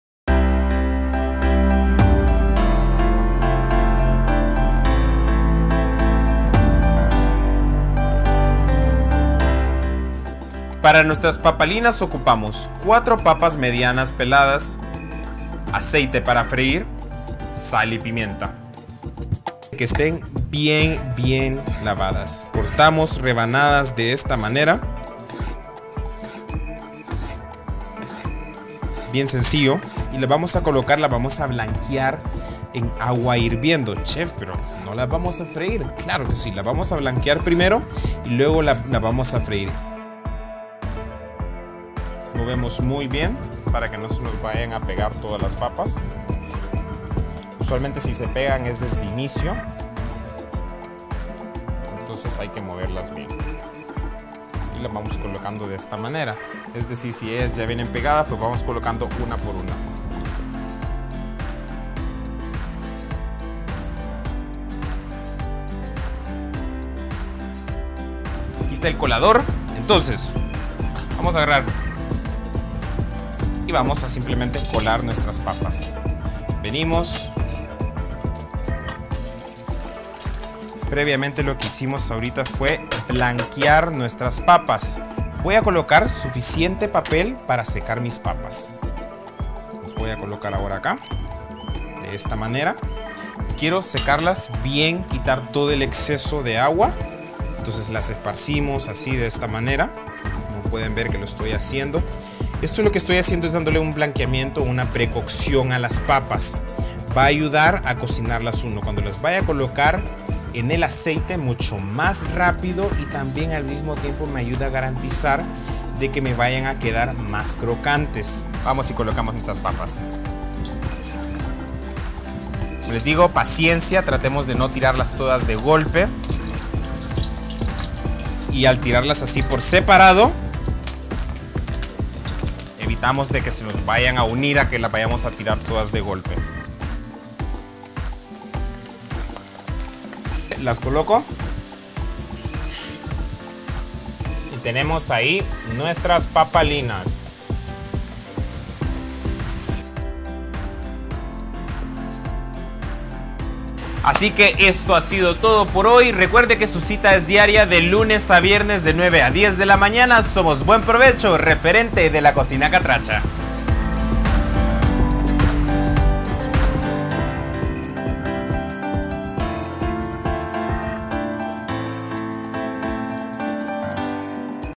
Papalinas sazonadas | Recetas fáciles y rápidas de hacer | Buen Provecho
Las papalinas sazonadas no son más que unas papas cortadas en rodajas finas, condimentadas al gusto y fritas en abundante aceite caliente.